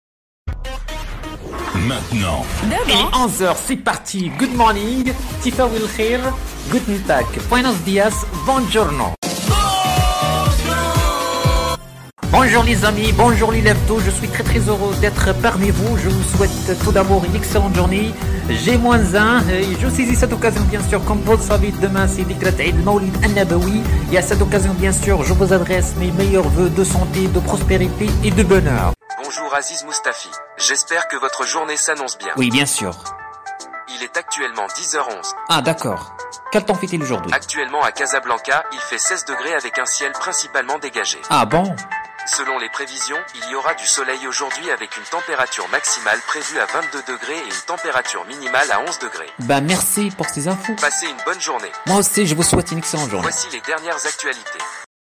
Podcast pour souhaiter une bonne fête de l'Aïd Mawlid Annabaoui (made with Spreaker)
Bonjour les lèves-tôt j'espère que votre journée s'annonce bien !

Comme vous le savez demain on va célébrer la fête de aid mawlid nabawi à cette occasion je vous souhaite bien sûr que de la réussite et tout le bonheur du monde.